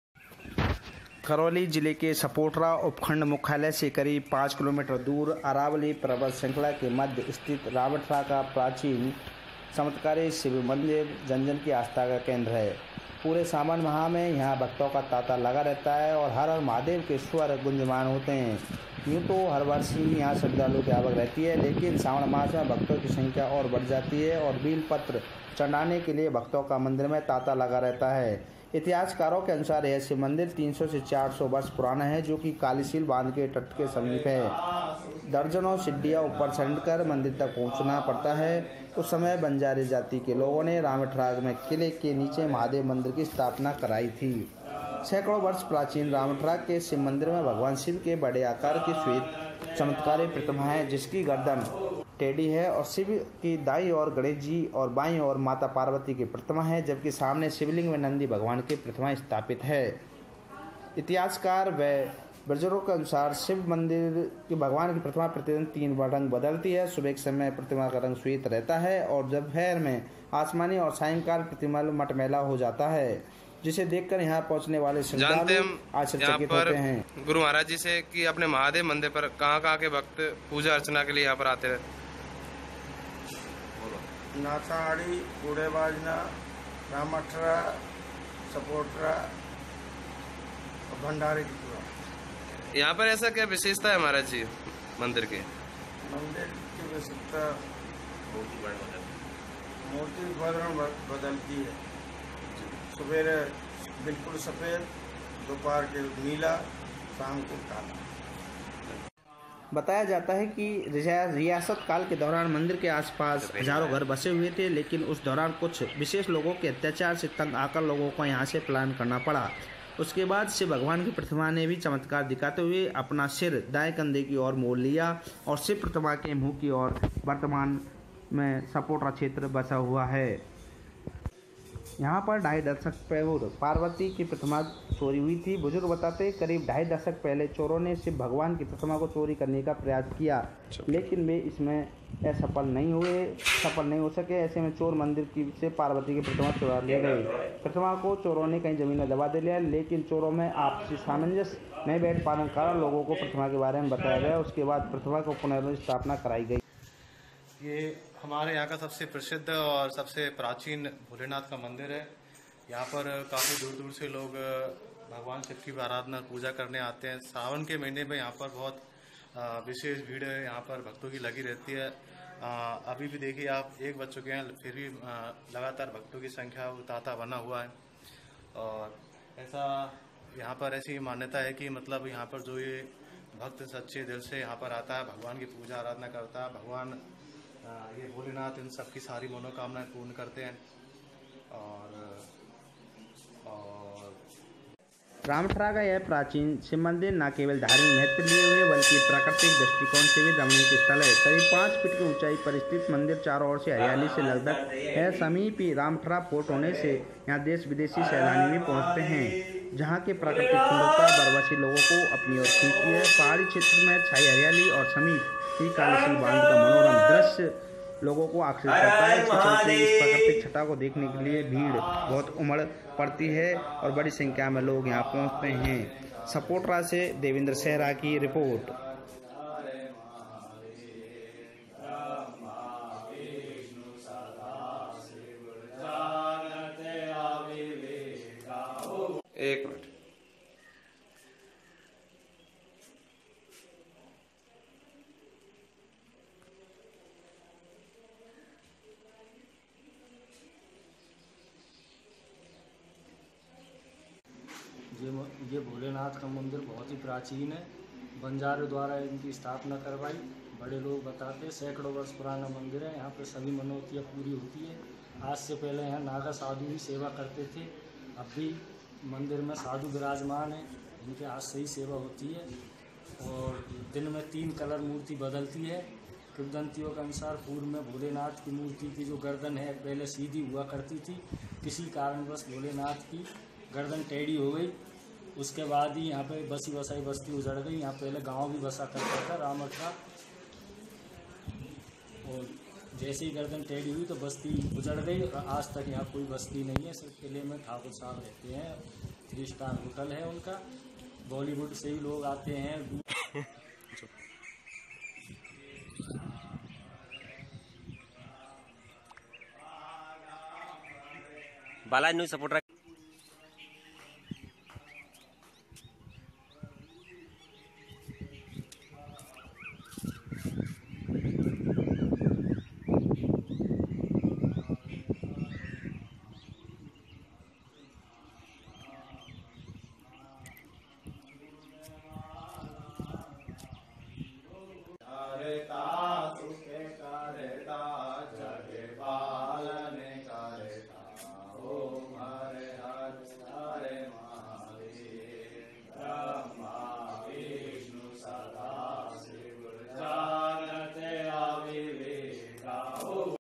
भगवान शिव का चमत्कार ऐसा कि प्रतिमा का दिन में तीन बार बदलता है रंग सपोटरा के रामठरा का प्राचीन
भगवान शिव का चमत्कार ऐसा कि प्रतिमा का दिन में तीन बार बदलता है रंग
सपोटरा के रामठरा का प्राचीन शिव मंदिर
श्रद्धालुओं का लगता है तांता, मनोकामना होती है पूरी

सपोटरा-  जिले के सपोटरा उपखण्ड मुख्यालय से करीब पांच किलोमीटर दूर अरावली पर्वत शृंखला के मध्य स्थित रामठरा का प्राचीन चमत्कारिक शिव मन्दिर जन-जन की आस्था का केन्द्र है। पूरे सावन माह में यहां भक्तों का तांता लगा रहता है और हर-हर महादेव के स्वर गुंजायमान होते हैं। यूं तो वर्षभर ही यहां श्रद्धालुओं की आवक रहती है, लेकिन श्रावण मास में भक्तों की संख्या और बढ़ जाती है। बिल्व पत्र चढ़ाने के लिए भक्तों का मंदिर में तांता लगा रहता है। इतिहासकारों के अनुसार यह शिव मंदिर करीब 350-400 वर्ष प्राचीन है, जो कालीसिल बांध के तट के समीप स्थित है। दर्जनों सीढिय़ां ऊपर चढ़कर मंदिर तक पहुंचना पड़ता है। उस समय बंजारा जाति के लोगों ने रामठरा में किले के नीचे महादेव मन्दिर की स्थापना कराई थी

शिव की अद्धुत चमत्कारिक प्रतिमा-

  सैंकड़ों वर्ष प्राचीन रामठरा के शिव मन्दिर में भगवान शिव की बड़े आकार की श्वेत चमत्कारिक प्रतिमा है, जिसकी गर्दन टेढ़ी है। शिव के दाई ओर गणेशजी और बांयी ओर माता पार्वती की प्रतिमा है, जबकि सामने शिवलिंग व नंदी भगवान की प्रतिमाएं स्थापित हैं। इतिहासकार व बुुर्जुगों के अनुसार शिव भगवान की प्रतिमा प्रतिदिन तीन वर्ण बदलती है। सुबह के समय प्रतिमा का रंग श्वेत रहता है, जबकि दोपहर में यह आसमानी हो जाती है। सायंकाल प्रतिमा मटमेले रंग में नजर आती है, जिसे देख यहां पहुंचने वाले श्रद्धालु भी आश्चर्यचकित हो उठते हैं।

ऐसे मुड़ी प्रतिमा की गर्दन

बताया जाता है कि रियासतकाल के दौरान मंदिर के आसपास हजारों घर बसे हुए थे, लेकिन उस दौरान कुछ विशेष लोगों के अत्याचारों से तंग आकर लोगों को यहां से पलायन करना पड़ा। उसके बाद शिव भगवान की प्रतिमा ने भी चमत्कार दिखाते हुए अपना सिर दांऐ कंधे की ओर मोड़ लिया। शिव प्रतिमा के मुंह की ओर वर्तमान मे सपोटरा क्षेत्र बसा हुआ है।

ढाई दशक पूर्व पार्वती की प्रतिमा हुई थी चोरी

  बुजुर्ग बताते हैं कि करीब ढाई दशक पहले चोरो ने शिवभगवान की प्रतिमा को चोरी करने का प्रयास किया, लेकिन वे इसमें सफल नहीं हो सके। ऐसे में चोर मंदिर से पार्वती की प्रतिमा को चुरा ले गए।  प्रतिमा को चोरों ने कहीं जमीन में दबा दिया। लेकिन चोरों में आपसी सामंजस्य नहीं बैठ पाने के कारण लोगों को प्रतिमा के बारे मे बताया गया। उसके बाद प्रतिमा की पुन: स्थापना कराई गई।

लुभाती है प्राकृतिक छठा

रामठरा का यह प्राचीन शिव मंदिर ना केवल धार्मिक महत्व लिए हुए है, बल्कि प्राकृतिक दृष्टिकोण से भी रमणीक स्थल है। करीब पांच फीट की ऊंचाई पर स्थित मंदिर चारों ओर से हरियाली से लकदक है। समीप ही रामठरा फोर्ट होने से यहां देशी-विदेशी सैलानी भी पहुंचते हैं। जहां की प्राकृतिक सुंदरता बरबस ही लोगों को अपनी ओर खींचती है। पहाड़ी क्षेत्र में छाई हरियाली और समीप ही कालीसिल बांध का मनोरम दृश्य लोगों को आर्कृषित करती है। इसके चलते इस प्राकृतिक छठा को देखने के लिए भी बड़ी संख्या में लोग यहां पहुंचते हैं।